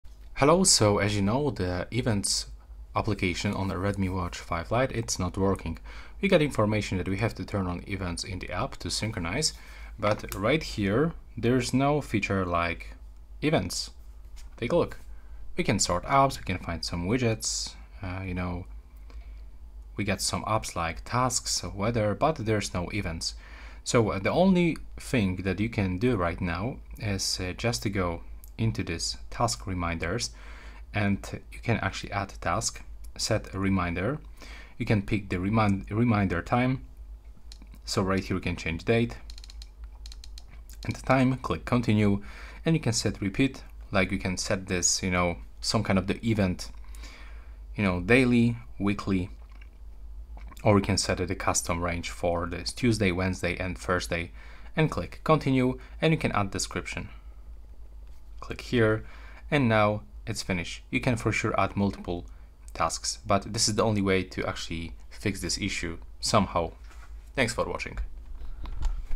Redmi Watch 5 Lite: How to Turn On Events
Learn how to turn on event notifications on your Redmi Watch 5 Lite to stay updated on important reminders. This guide walks you through the steps to enable event alerts, ensuring you never miss an appointment or special occasion. Follow along to customize your notification settings for a more organized experience. Discover how to make the most of your Redmi Watch 5 Lite with event reminders at your fingertips.

I'll do my best to reply promptly and assist you.